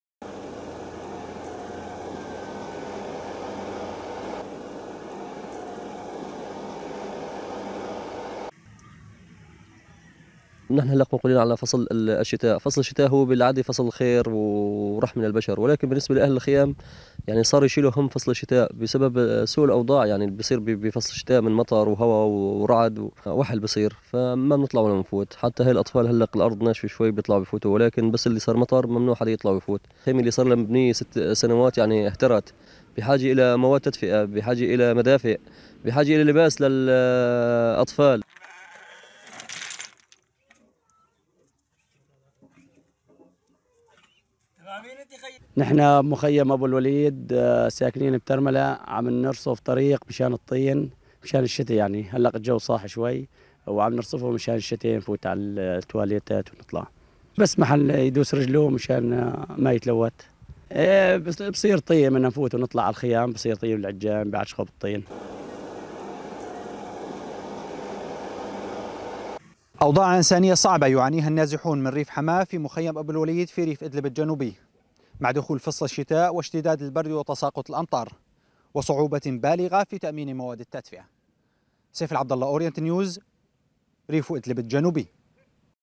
مع دخول فصل الشتاء..أوضاع إنسانية صعبة في مخيم أبو الوليد جنوب إدلب
يعاني النازحون من ريف حماة في مخيم أبو الوليد بترملا جنوب إدلب من سوء الأوضاع المعيشية، وشحِّ الخدمات الضرورية اللازمة للعيش الإنساني، حيث يعاني آلاف النازحين في المخيمات سوء الخيم، التي لا تمنع عنهم برد الشتاء. بالإضافة لنقص كبير في وسائل التدفئة، لا سيما مع دخول فصل الشتاء..

تقرير: سيف العبدلله